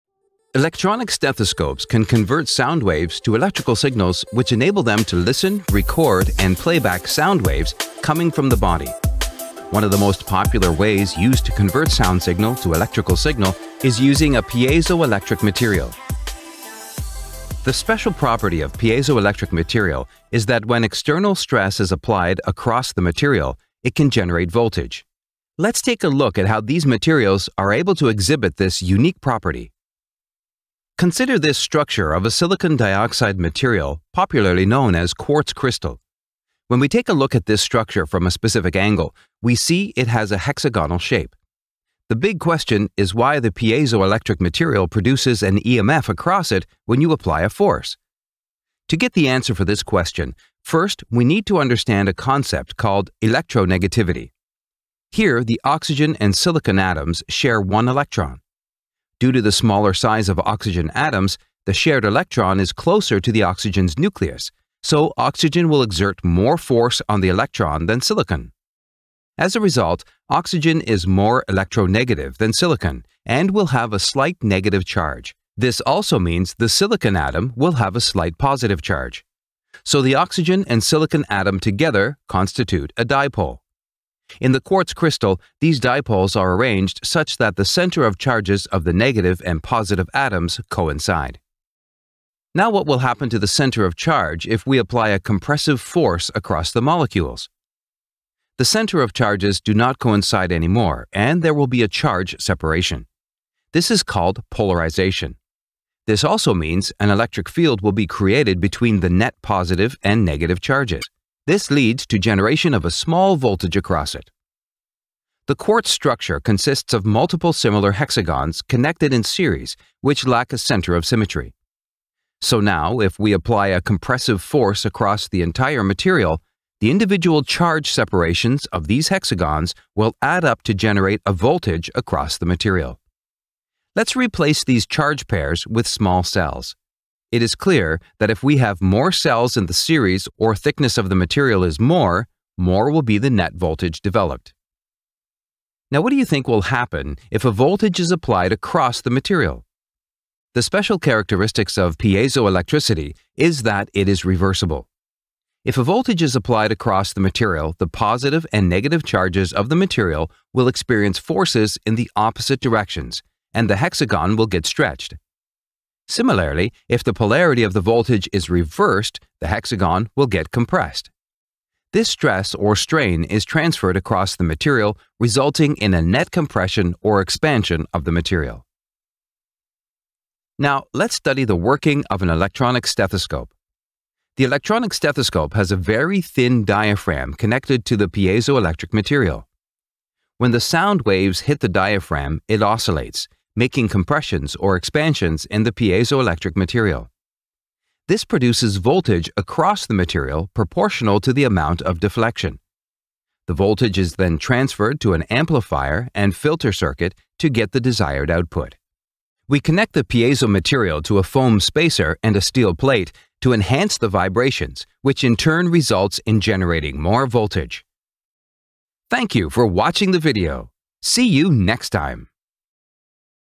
Understanding Piezoelectric effect!
Let's understnand the physics behind the piezoelectric materials in a detailed way.